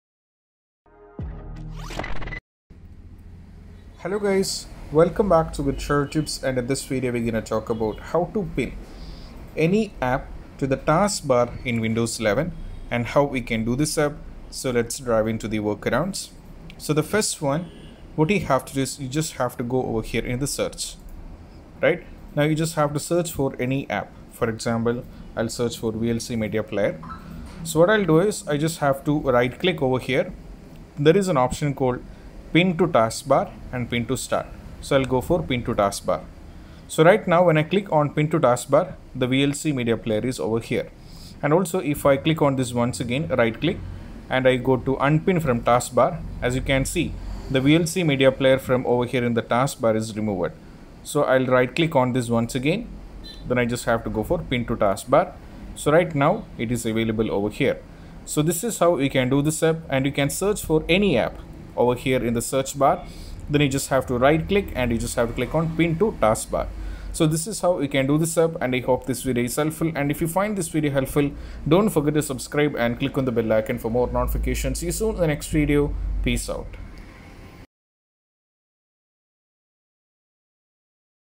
How to Pin Any App to the Taskbar in Windows 11 (Tutorial)
We spend so much time on our PC every day, browsing sites, engaging on social media, working, and using apps. As such, having the ability to boot up our most-used apps in a single click can be a convenient and time-saving luxury.
Windows has always let you pin apps to the taskbar for instant access, and Windows 11 is no different. However, Windows 11 now lets you pin files, folders, drives, and even websites to the taskbar too. So, how do you pin things to the Windows 11 taskbar?